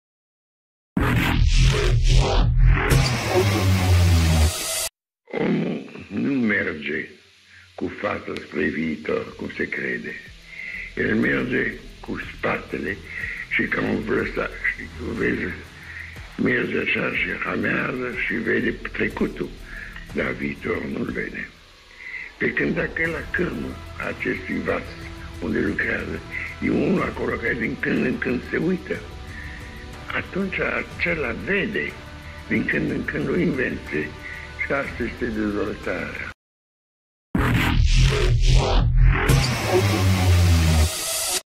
Video rar Henri Coanda "Omul nu merge cu fața spre viitor cum se crede"
Henri Marie Coandă a fost un academician și inginer aeronautic român, pionier al aviației, fizician, specialist în aerodinamică și mecanica fluidelor, inovator, inventator și descoperitor al efectului care îi poartă numele.